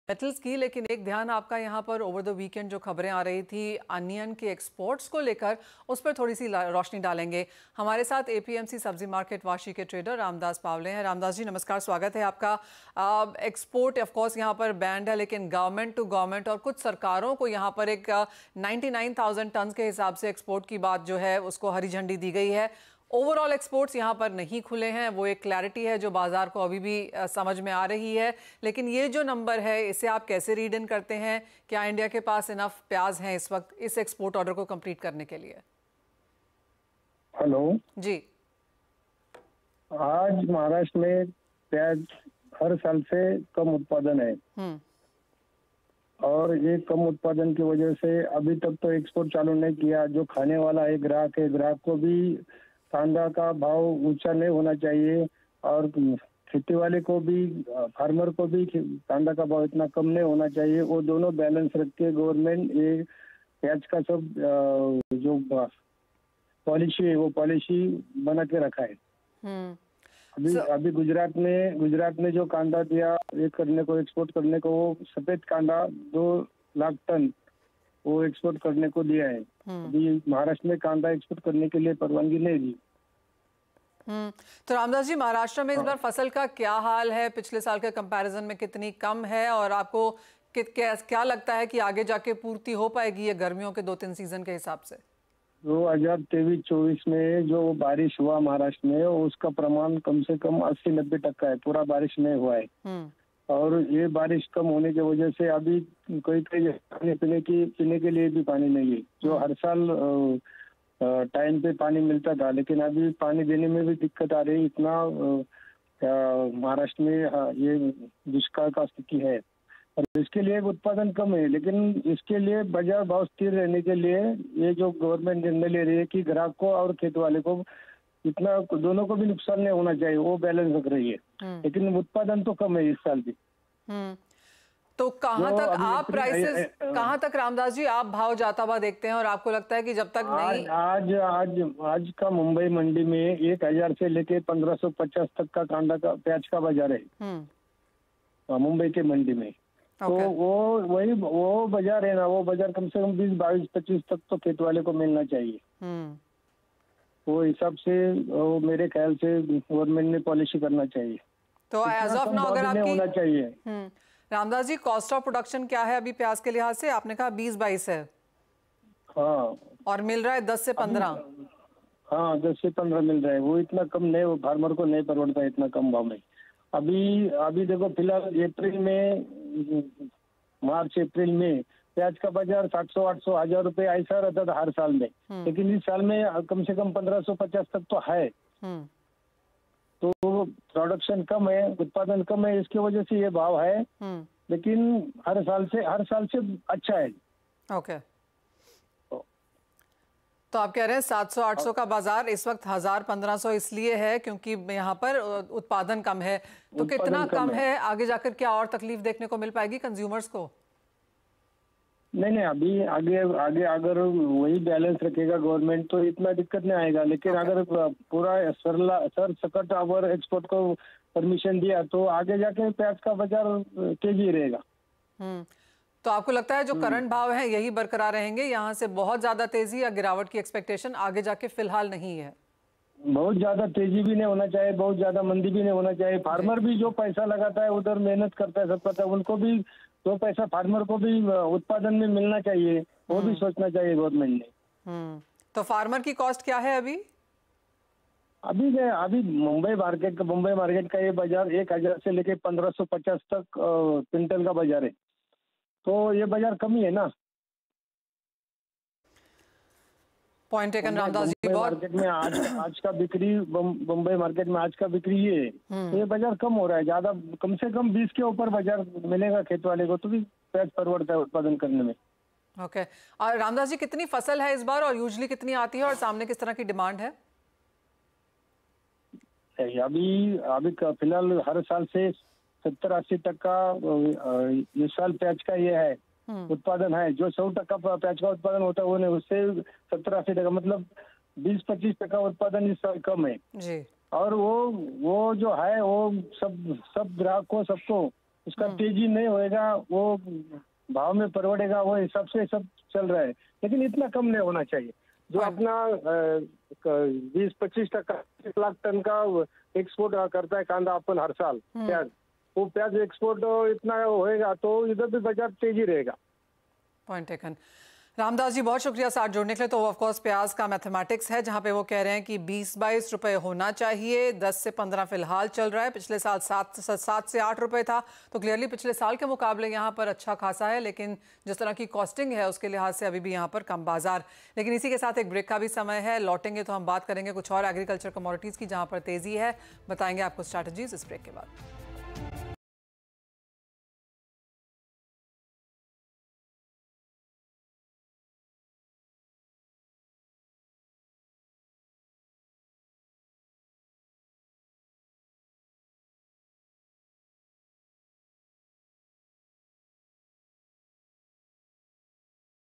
Onion Export Ban News | भारत में कम हुआ उत्पादन? आगे कहां तक जाएंगे दाम? | Onion MSP Rates
दूसरे देशों में प्याज एक्सपोर्ट का काम देख रही है नेशनल को-ऑपरेटिव एक्सपोर्ट्स (NCEL) ने ई-प्लैटफॉर्म्स के जरिए L1 कीमत पर घरेलू प्याज की खरीद की. इसके बाद इन 6 देशों की एजेंसी को NCEL ने प्याज की सप्लाई की है.

n18oc_money